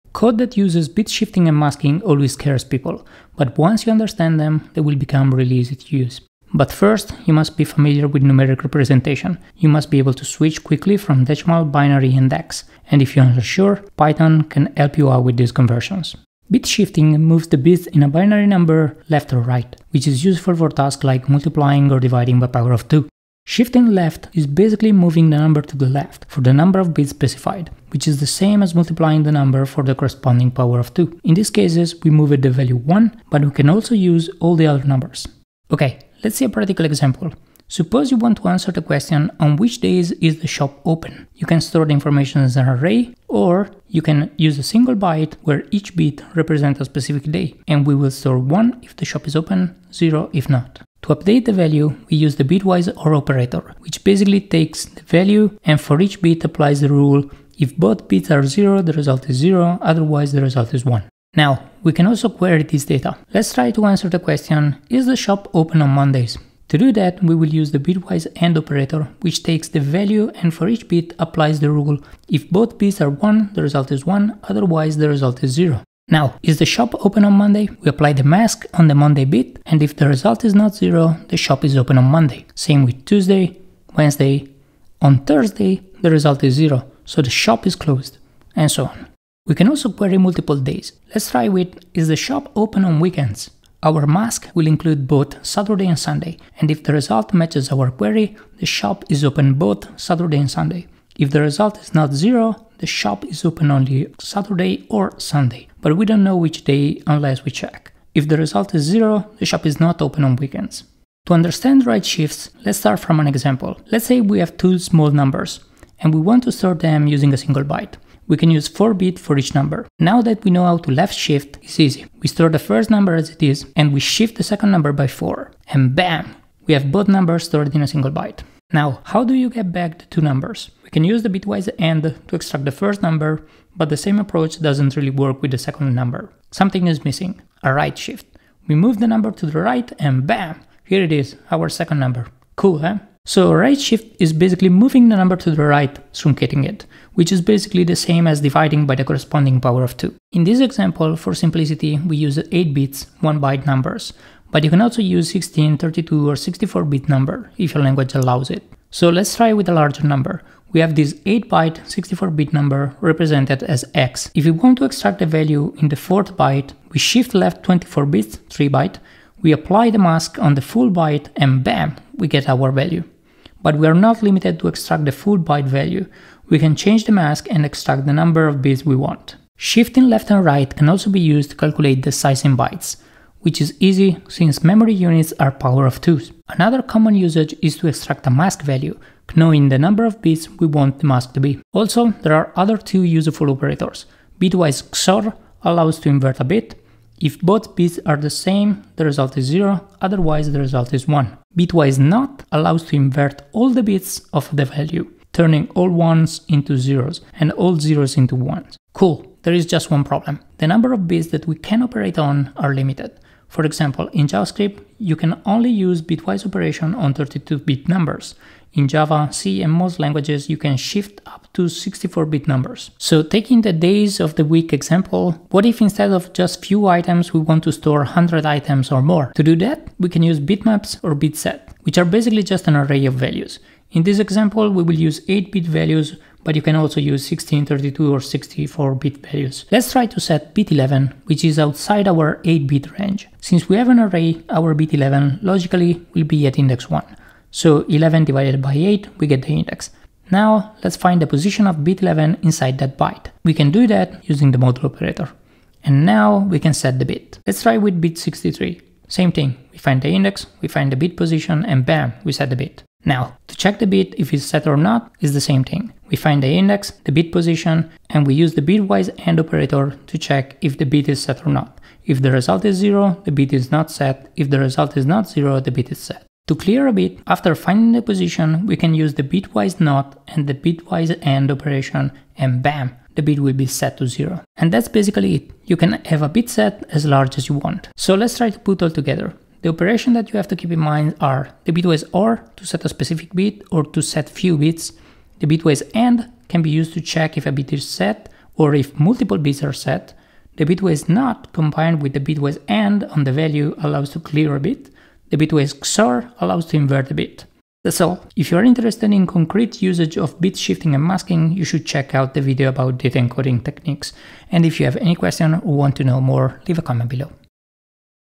Mastering Bitwise Operations: Shifting, Masking, and More!
Learn about Bit shifting, Bit Masking and the various Bitwise operations and don't be afraid of them!

Chapters:
0:00 Intro
0:07 Binary Representation
0:24 Left Shift
0:36 A Practical Example
1:52 Right Shift
3:00 Tricks
3:30 Bitmaps
4:48 Recap Bitwise Operations